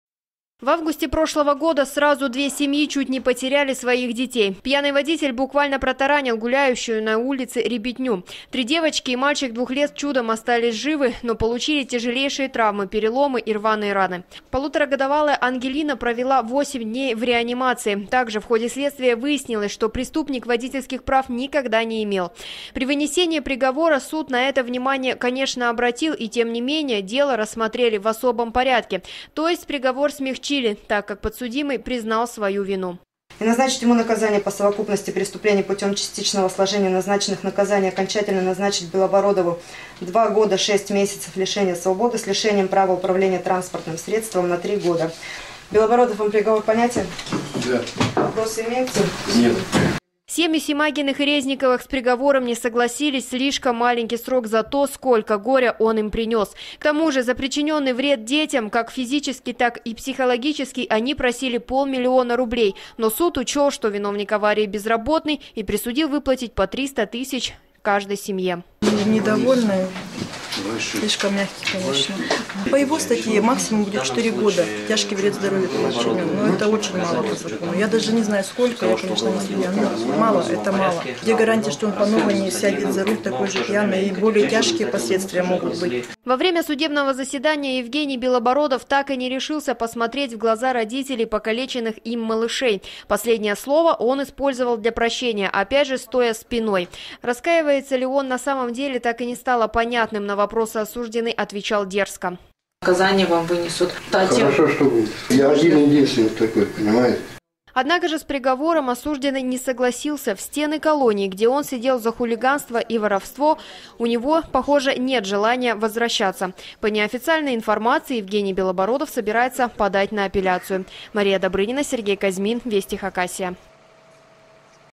2 года 8 месяцев тюрьмы за 4 искалеченных детей 29.01.216
Приговор суда не устроил ни родителей пострадавших, ни виновника пьяного ДТП. Сегодня в белоярском суде вынесен приговор жителю Алтайского района, который будучи пьяным, пытался скрыться от полицейских , и сбил четверых детей. Жуткое ДТП произошло летом в селе Изыхские Копи. Какое наказание заслужил подсудимый?